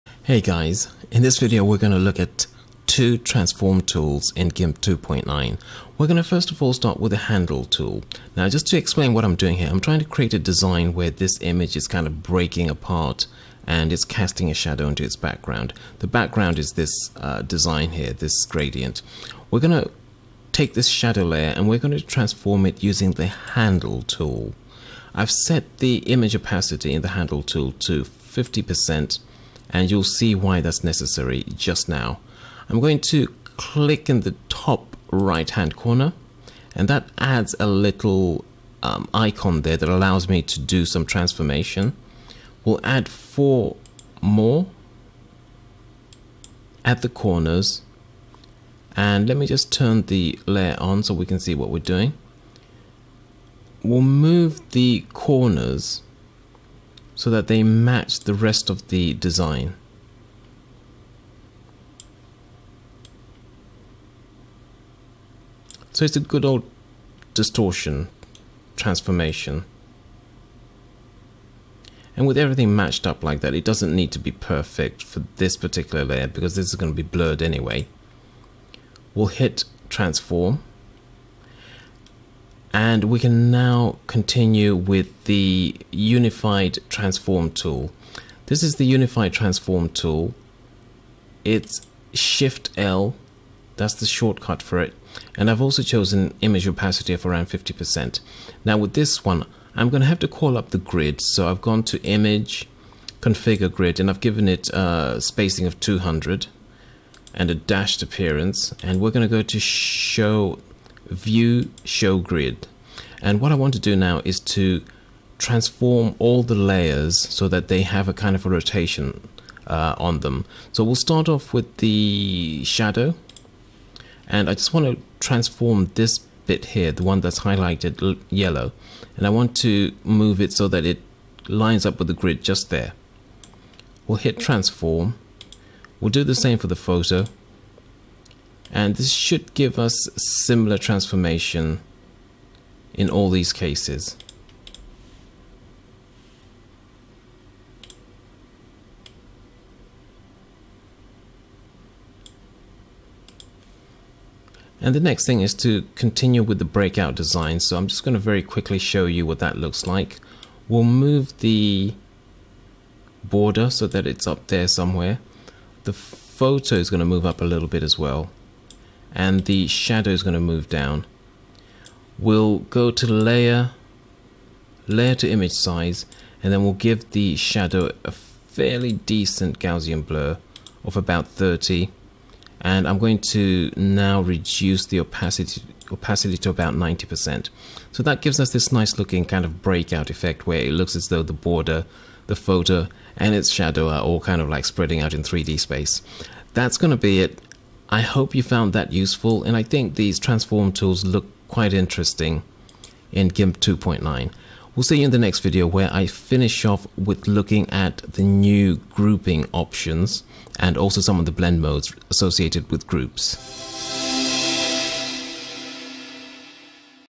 Gimp 2.10 'New Featurettes' 3/4 - Handle and Unified Transform Tools
Two new powerful transform tools are available for Gimp 2.9 and hopefully these will see it into the Gimp 2.10 release.